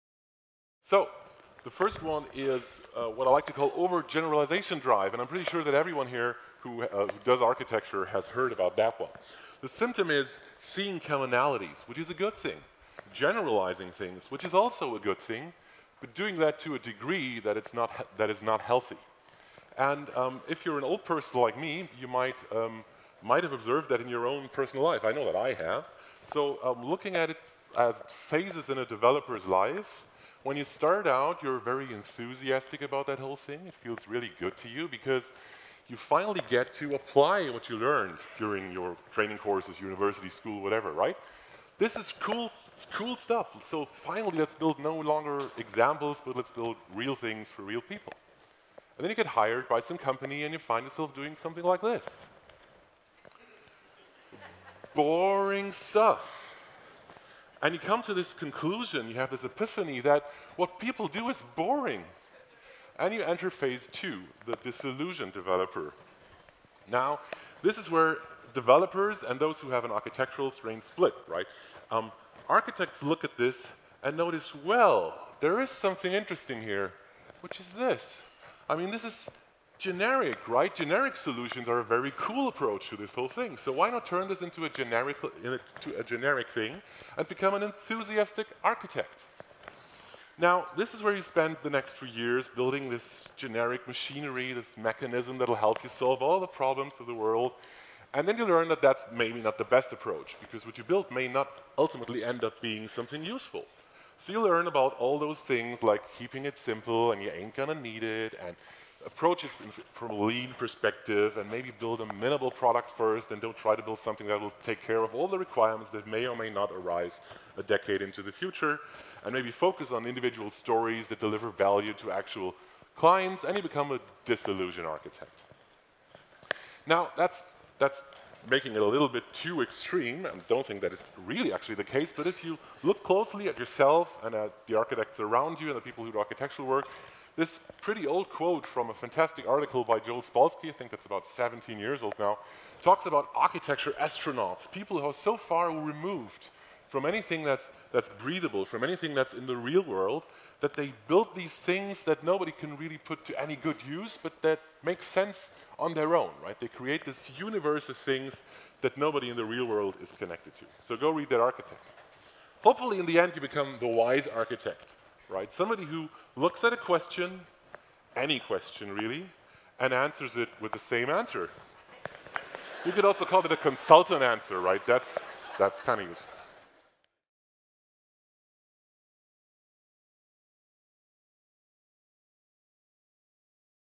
Why software architects fail and what to do about it - Stefan Tilkov (INNOQ)
We’ve all seen them: ambitious projects that started out with grand visions and ended up as costly lessons in what not to do, leaving behind the ruins of promising paradigms, technologies, tools, and careers. But why do architecture approaches sometimes hurt instead of providing value? Why has “architect” become a negative term for some people? And what can we do to improve our own work? Stefan Tilkov looks at some of the most common pitfalls that ensure you’ll come up with a disaster and explains how they can be avoided.